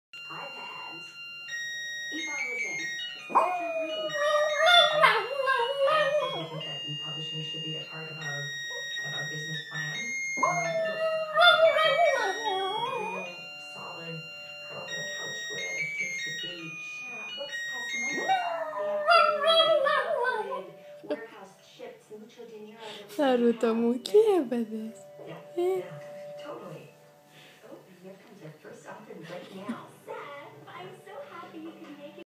yorkie puppy howl
Naruto goes crazy when the phone rings! MUST SEE! SO FUNNY!